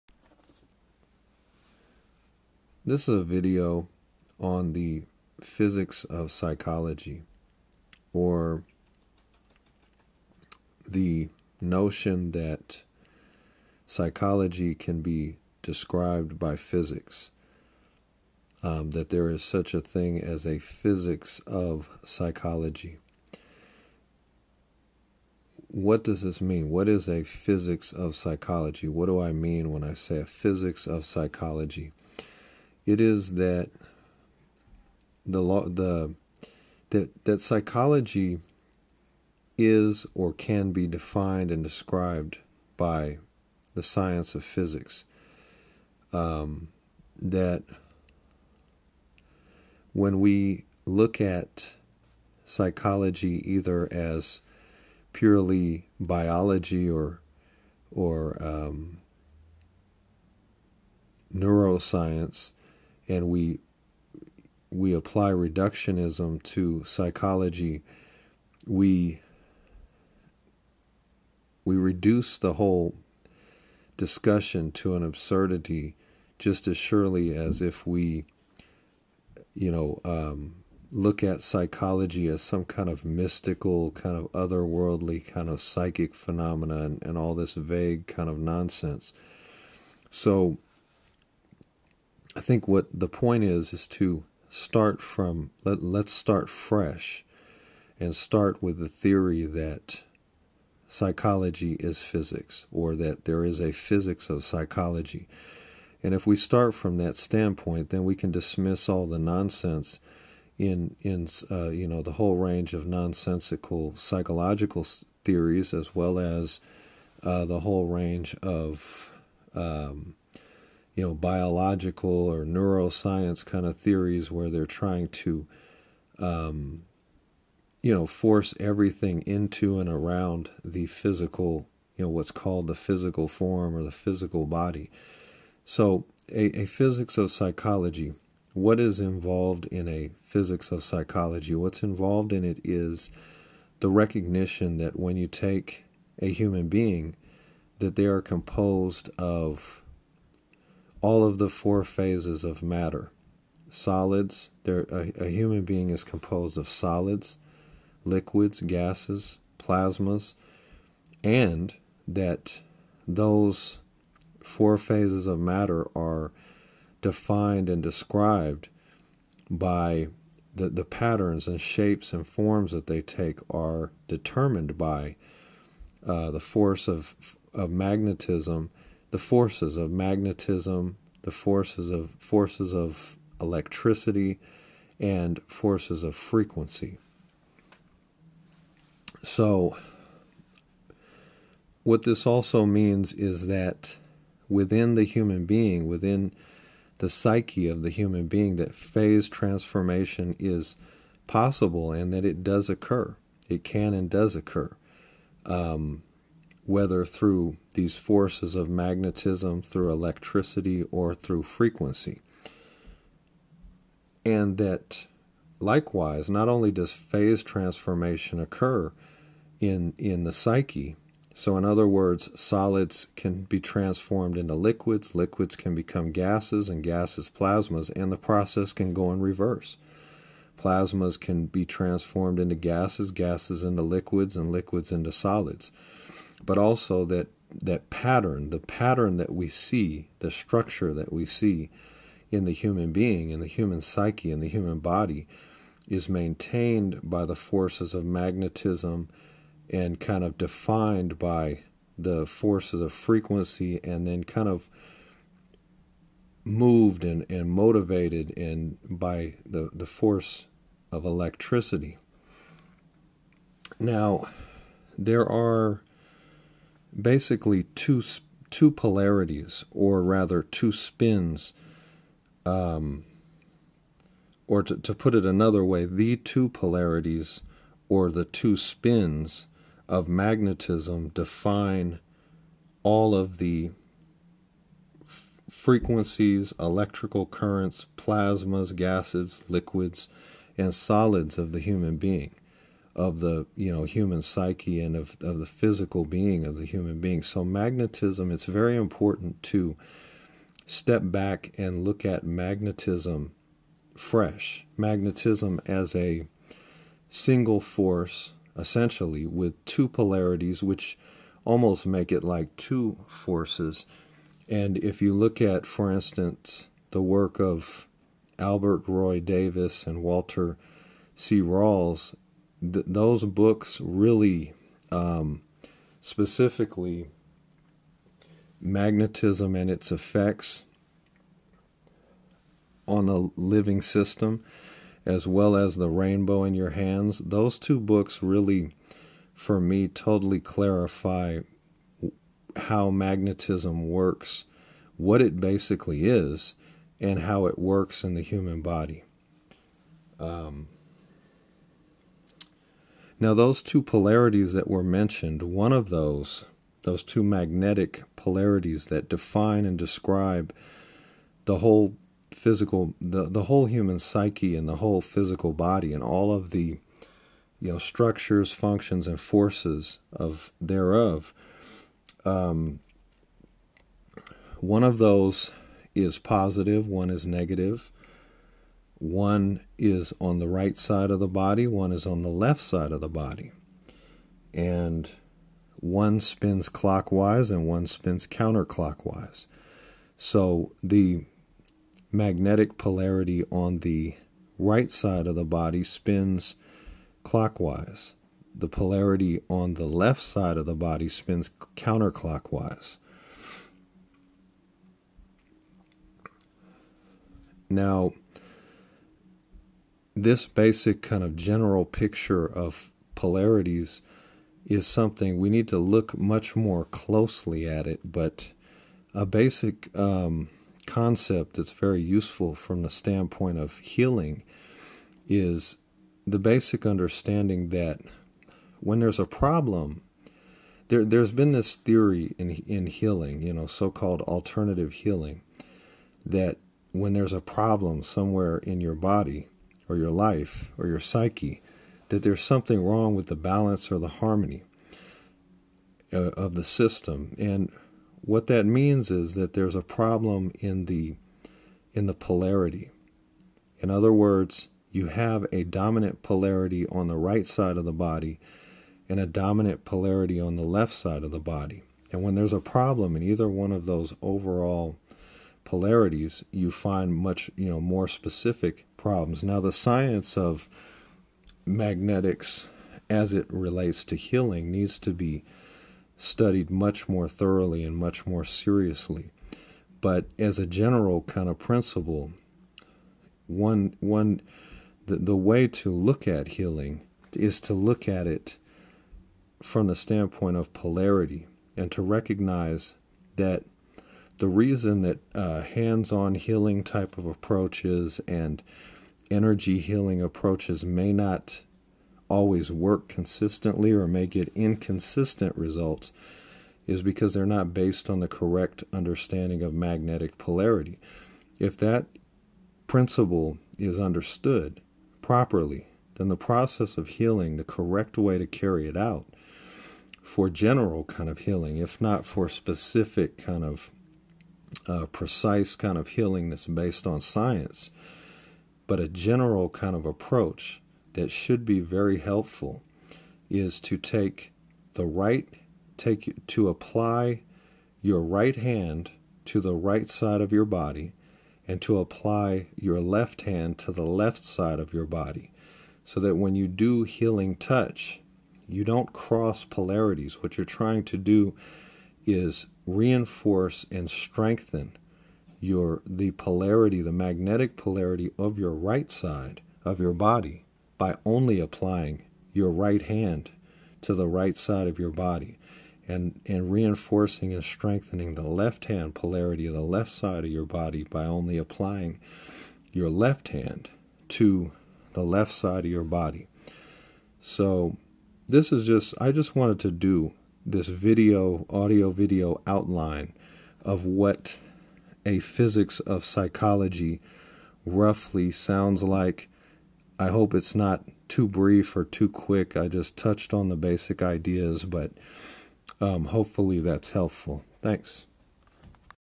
Physics of Psychology
This audio-video provides an outline of the theory that psychology is physics or that the science of physics can be utilized to understand, to define and to describe psychology. Reductionism is absurd and mystifying conceptions of psychic phenomena are irrelevant. It is much simpler to allow the facts of psychological experience to speak for themselves, rather than attempting to analyze every subjective experience through a conceptual filter that may be inaccurate.

A physics of psychology means that we approach psychic experience fresh and take it at face value, not making unnecessary assumptions or presumptuous conclusions. The human being is composed of Solids, Liquids, Gases and Plasma's that are patterned, formed and moved by Magnetism, Electricity and Frequency. Likewise, phase transition can be brought about through Frequency, Electricity and/or Magnetism. In the human psyche there is no such thing as a "placebo" or some vague, indefinable "psychosomatic" influence, as there is only physics involving, Solids, Liquids, Gases, Plasma's, Frequencies, Electricity and Magnetism.

All psychic/psychological experiences or phenomena become explicable when they are described accurately as what they are rather than being denied or dismissed. Reductionism tries to do the impossible and most "subtle energy" theories make psychic phenomena inexplicable. Within a physics of psychology every human experience is comprehensible, measureable and can be described precisely.

As Albert Roy Davis and Walter C. Rawls have shown in their book, "Magnetism and Its Effects on the Living System," magnetism is the fundamental force of nature and it possesses two polarities or spins. The two polarities/spins of magnetism define all patterns of frequency, electrical current, plasma, gas, liquid or solid. The currents of magnetism are positive/negative and spin clockwise and counter-clockwise. These magnetic spins/polarities are self-evident in the left side and in the right side of the human body.

Davis and Rawls also suggest in their book, "The Rainbow In Your Hands," that the right palm and the left palm each carry the opposite polarity. Why are healers inconsistent in their results? The key to general self-healing seems to reside in one key principle: magnetic polarity. When healers attempt to heal others they generally use both hands, through touch and/or at a distance but this approach will get mixed results. The correct way to heal self or others is to apply the right hand to the right side of the body and the left hand to the left side of the body. The goal being to strengthen the dominant polarity by strengthening the clockwise spin on the right side and enhancing the counter-clockwise spin on the left side.